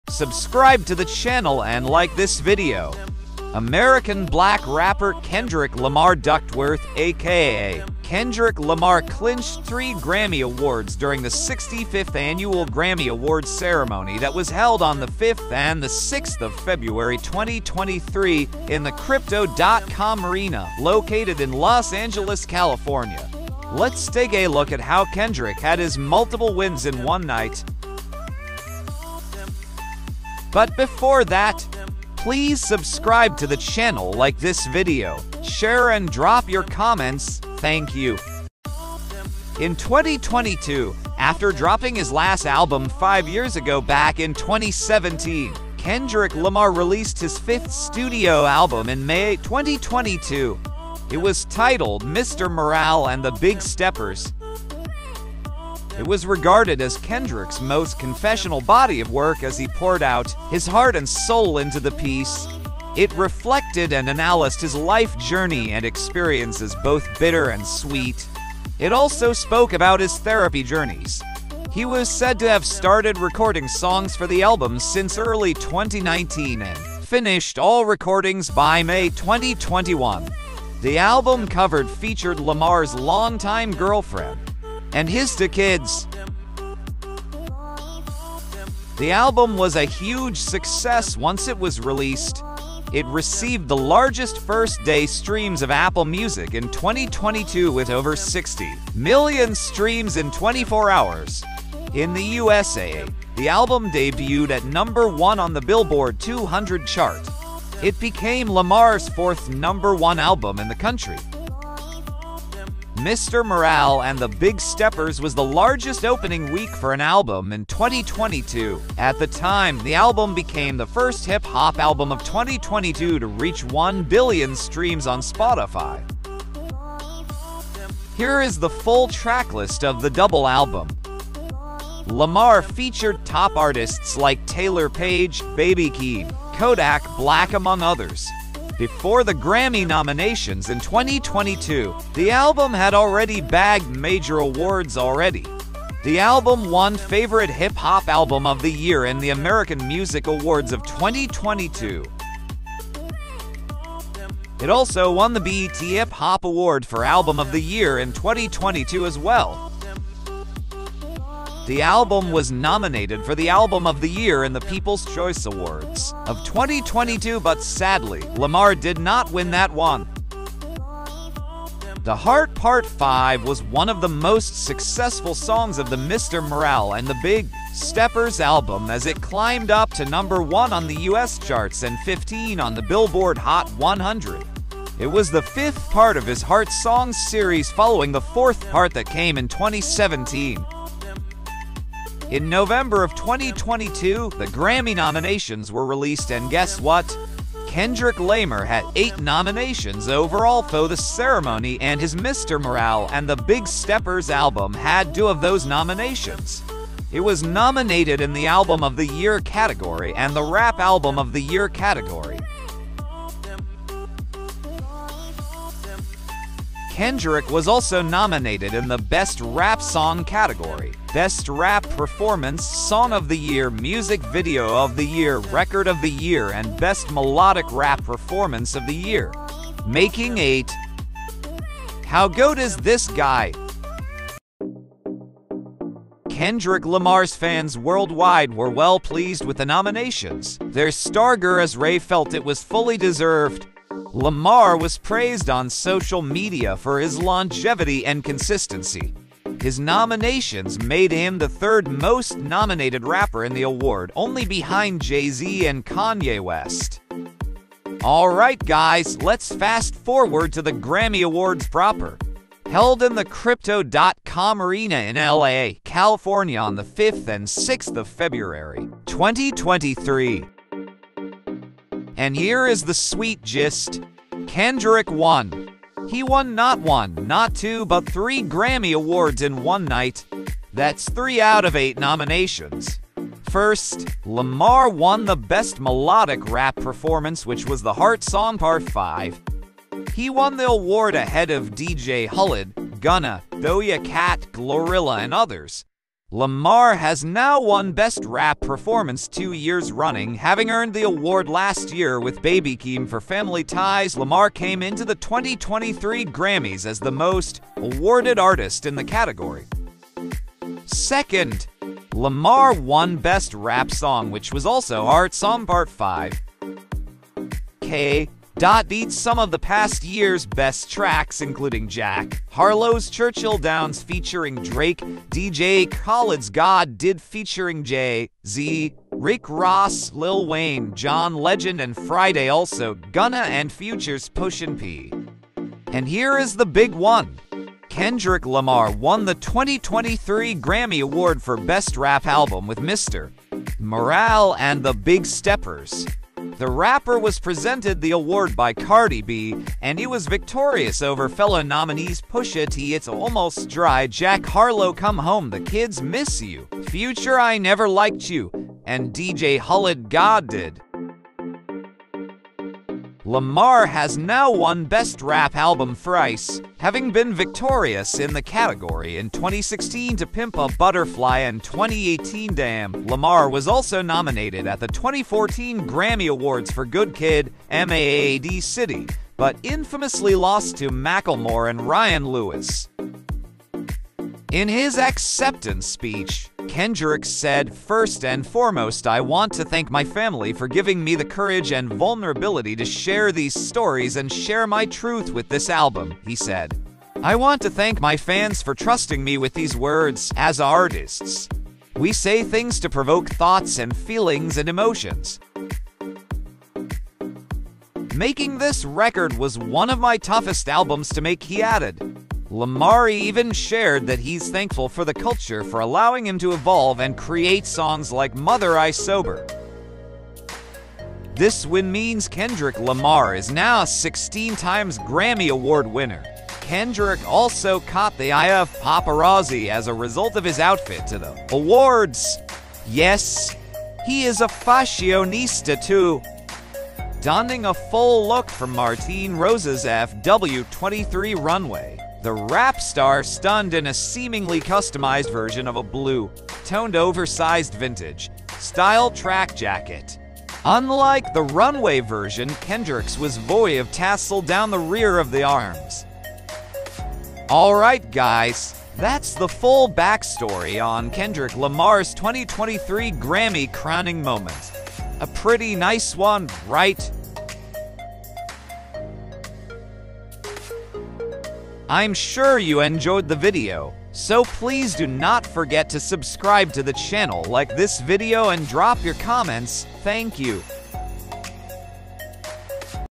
KENDRICK LAMAR: WHY HE KEEPS WINNING BEST RAP ALBUM AT THE GRAMMYS
Kendrick Lamar who is an American rapper and songwriter won 3 Grammy award in the just concluded Grammy.